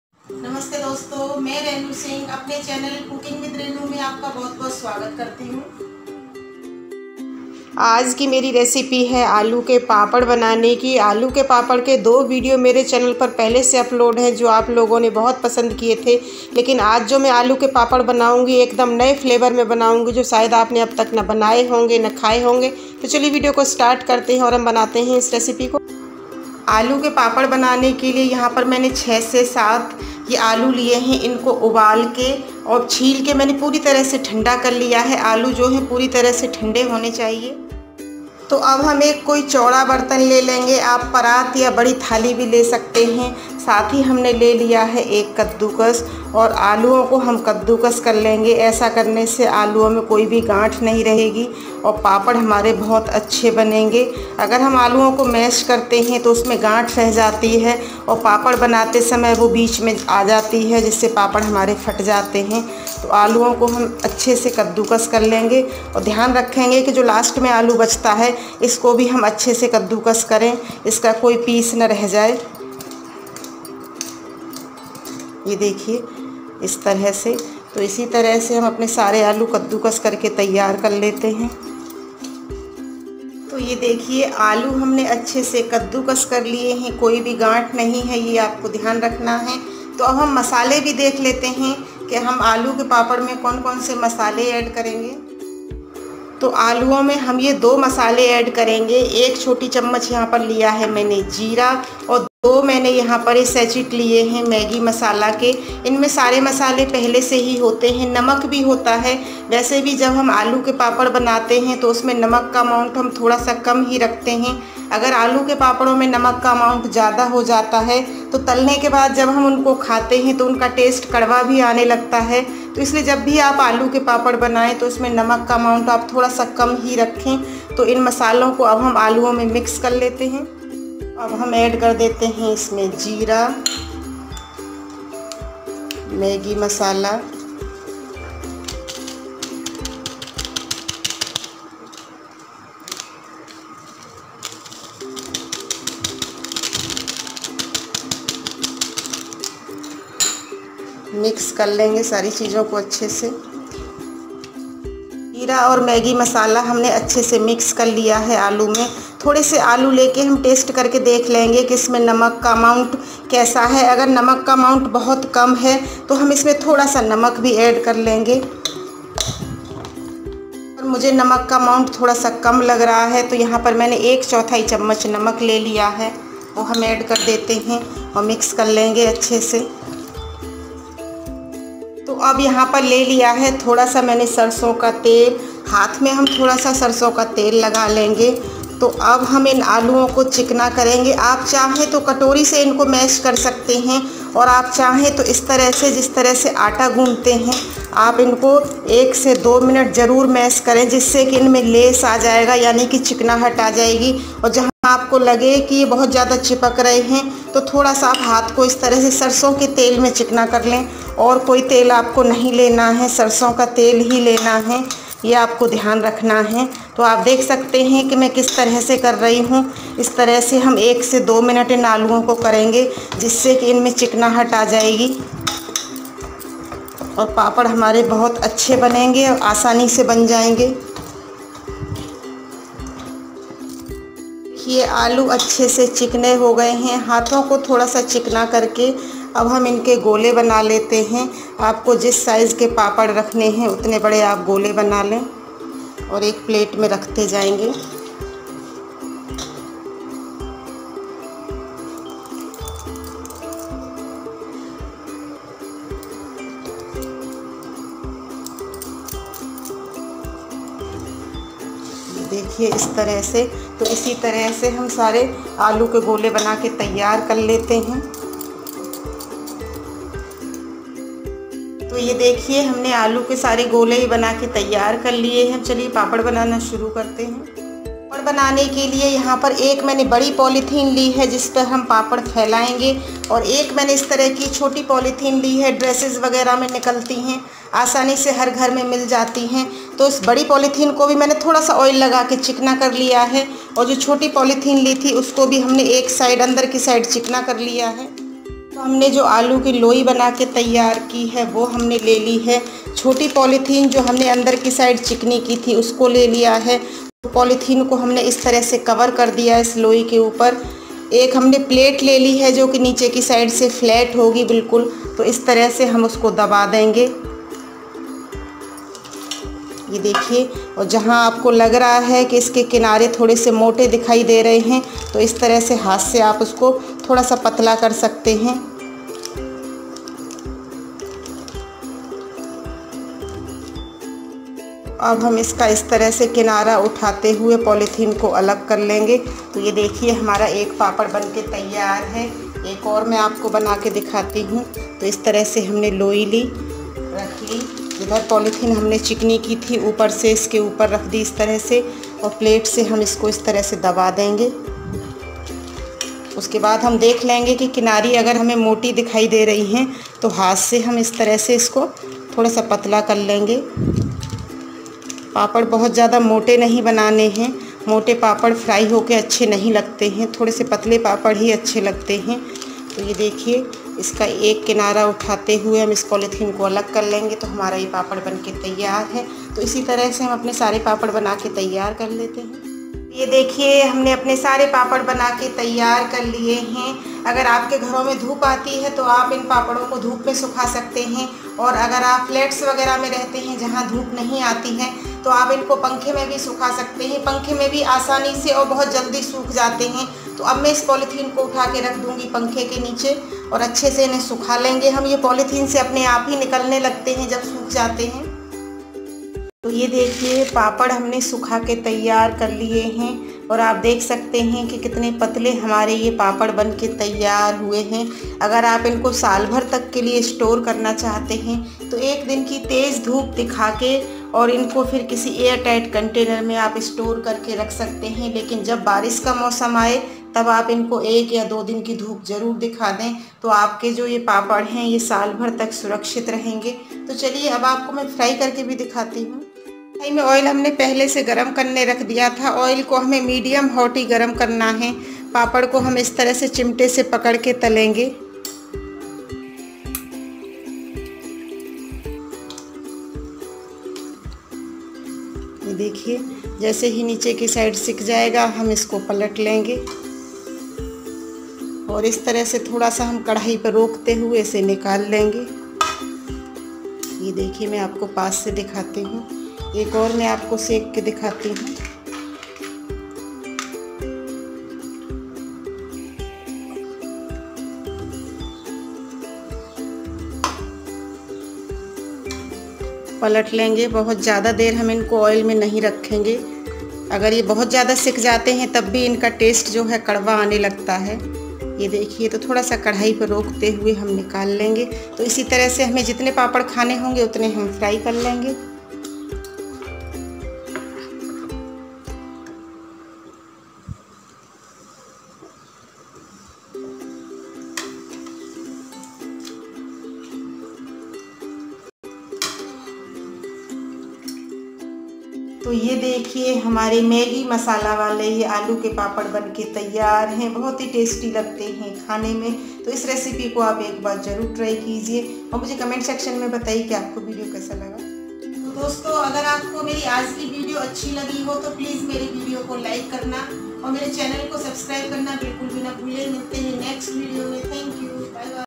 बिना धूप बनायें आलू के पापड़ एक नए फ्लेवर में।Homemade aloo papad recipe
Homemade aloo papad recipe by cooking with renu

Ingredients-
Boiled potatoes- 7
Maggie masala-2